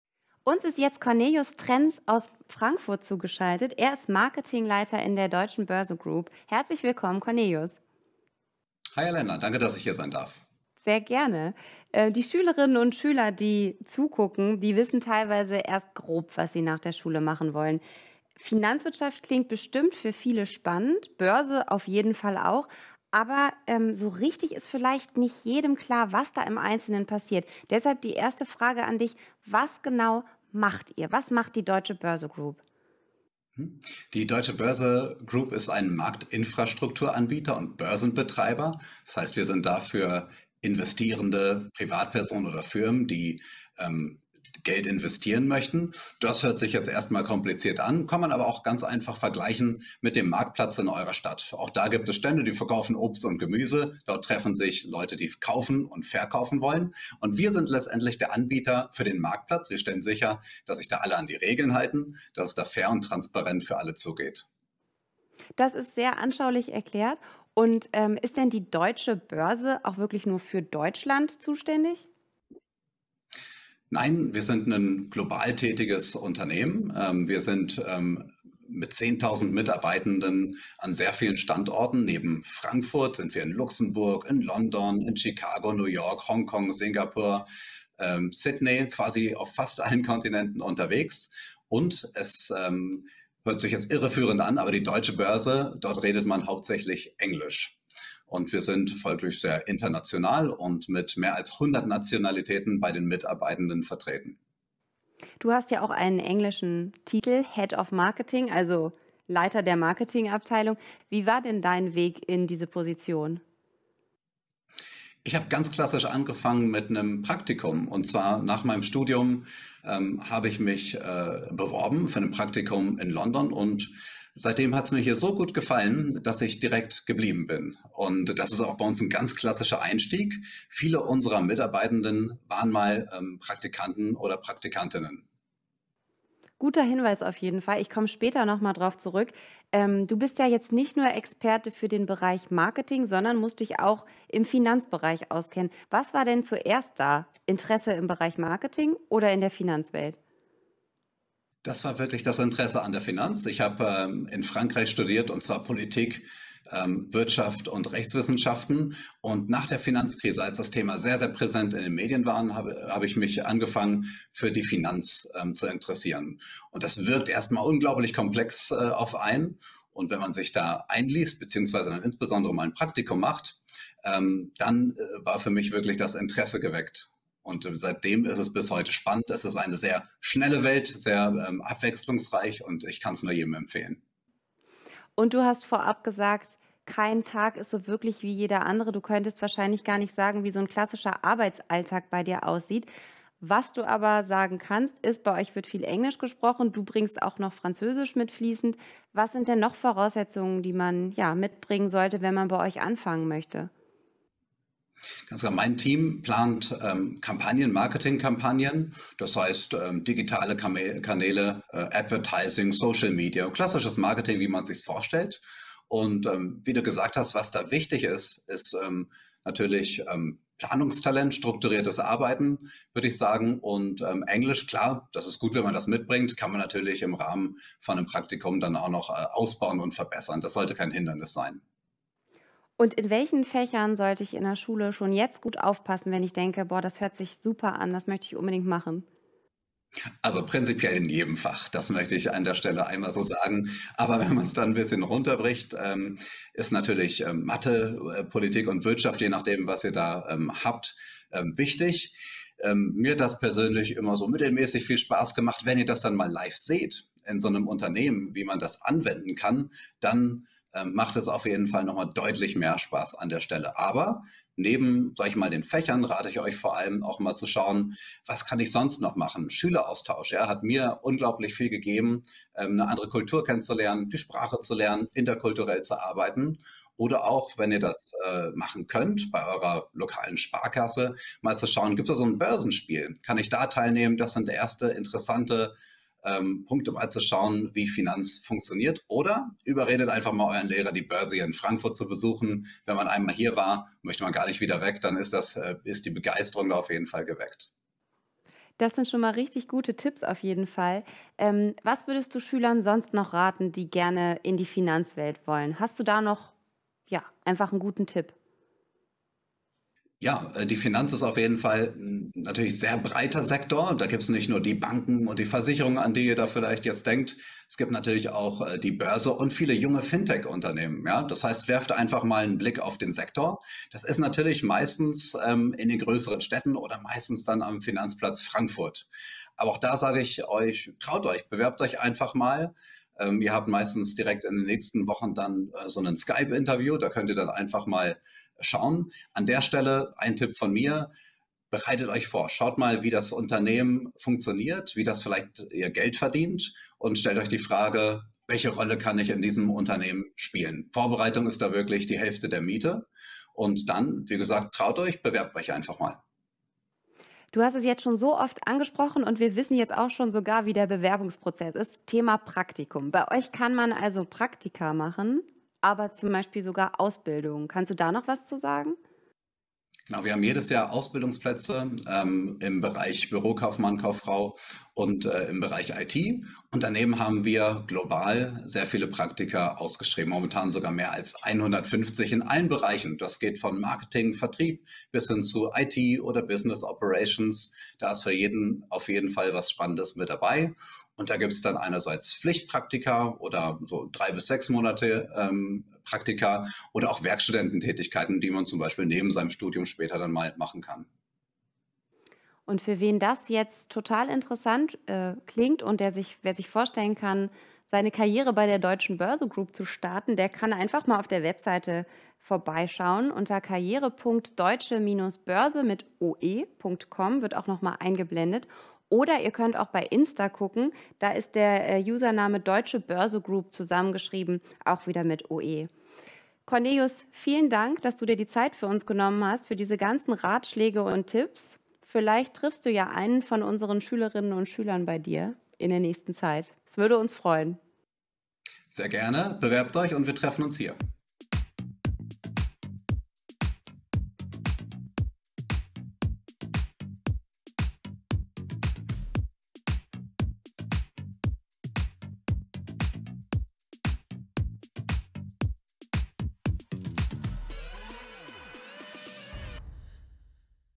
Money, Money, Money!? Im Gespräch: Cornelius Trenz, Deutsche Börse | Special Guest | What's Next?
Was genau macht man eigentlich an der Börse? Cornelius Trenz, Head of Marketing bei der Deutschen Börse Group, erklärt in einfach Worten, wofür sein Unternehmen zuständig ist. Aber Elena will es natürlich genauer wissen: Wie hat es Cornelius in seinen Job geschafft und was muss man mitbringen, wenn man sich dafür interessiert? All das und noch viel mehr bespricht sie mit ihm in diesem Interview.

What's Next? Berufe. Ausbildung. Studium. Job.